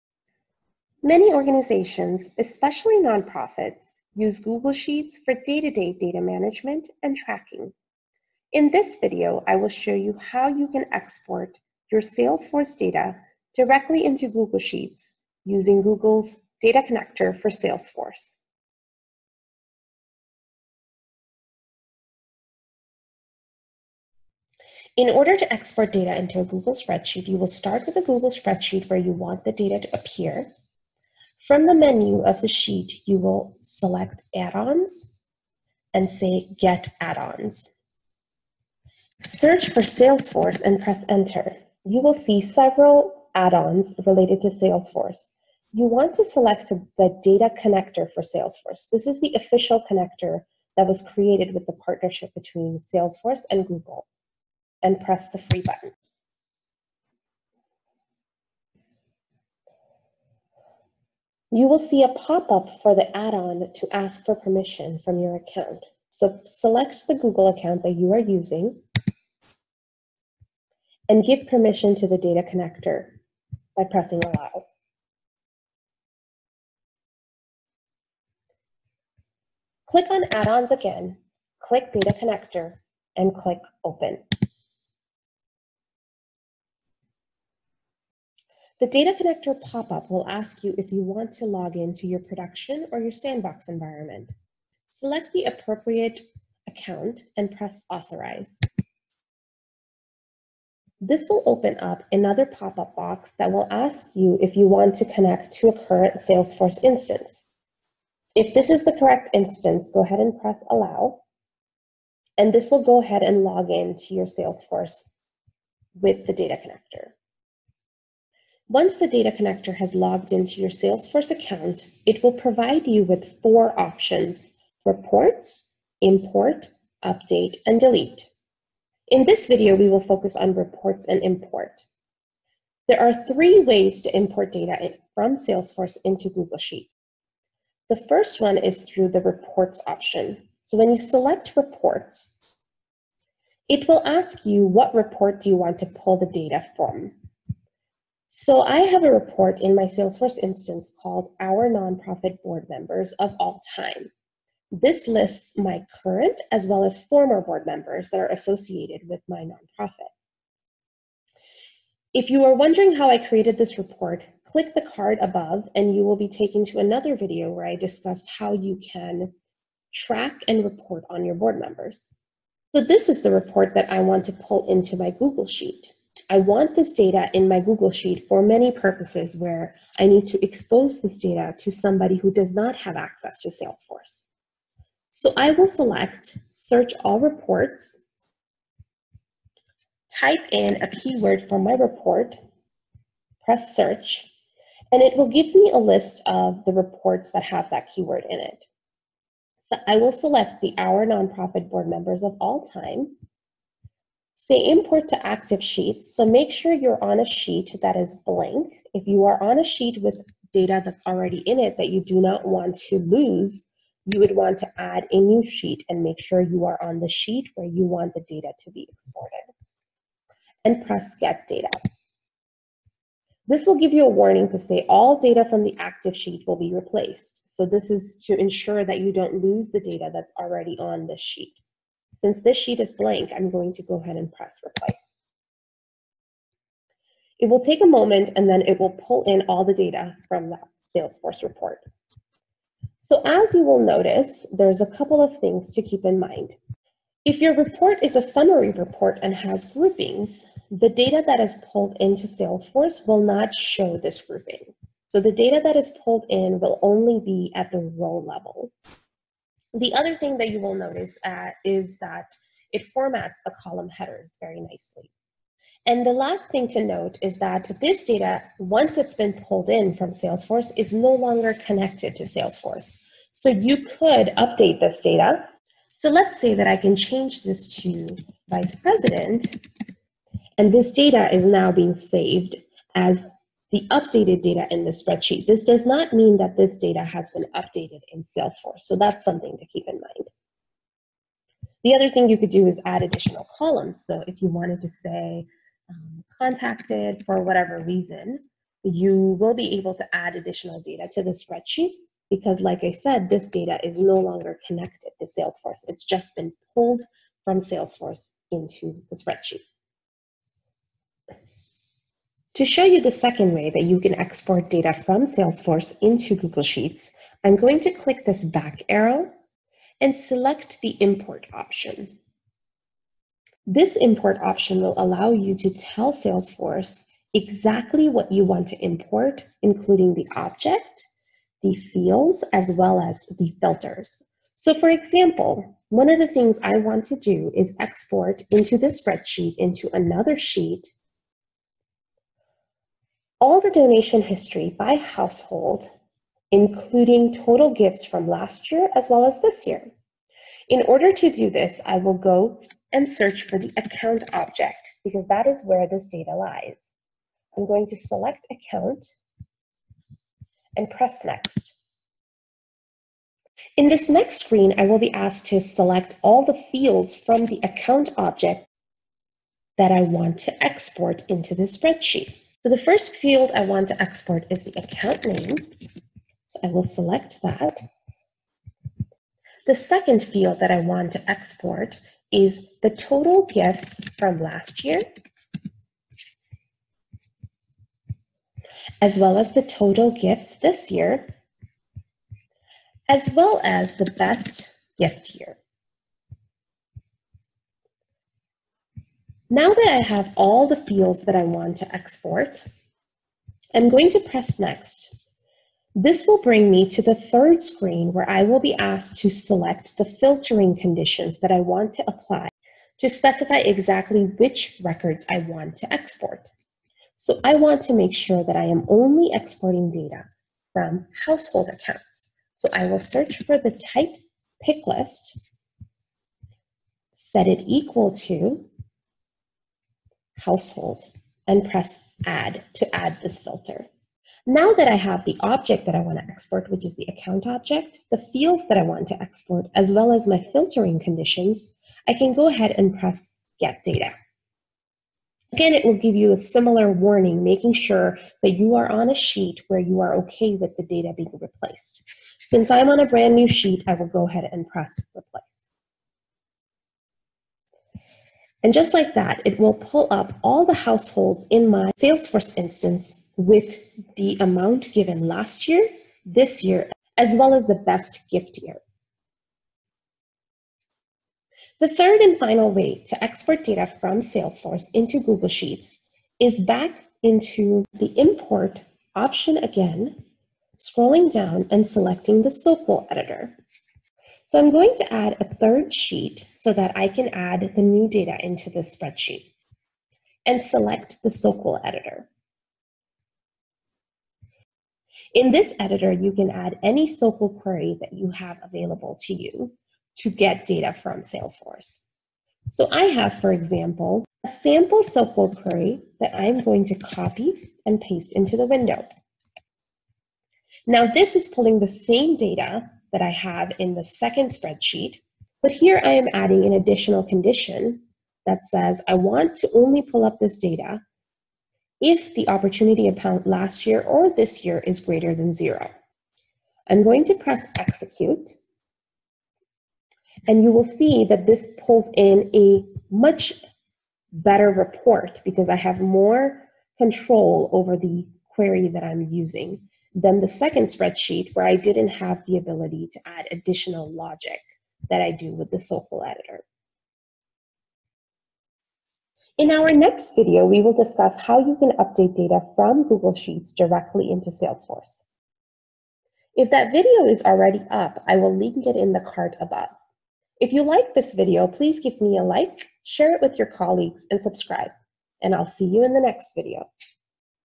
Export Salesforce Data into Google Sheets
Many organizations, especially nonprofits, use google sheets for day to day data management and tracking. In this video I will show you how you can export Salesforce data directly into Google sheets.

You will learn how to:
1. Install the Data Connector for Salesforce add-on in Google Spreadsheets
2. Export Salesforce report data into Google Sheets
3. Export data from a Salesforce objects into Google Sheets
4. Export the results of a SOQL query into Google Sheets